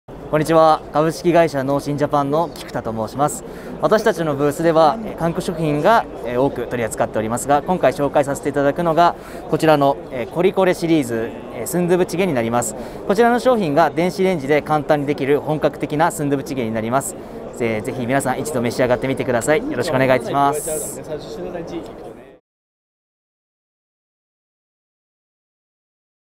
[第57回スーパーマーケット・トレードショー2023] コリコレシリーズ スンドゥブチゲ - 株式会社農心ジャパン
株式会社農心ジャパンは第57回スーパーマーケット・トレードショー2023にて、コリコレシリーズ スンドゥブチゲを出展。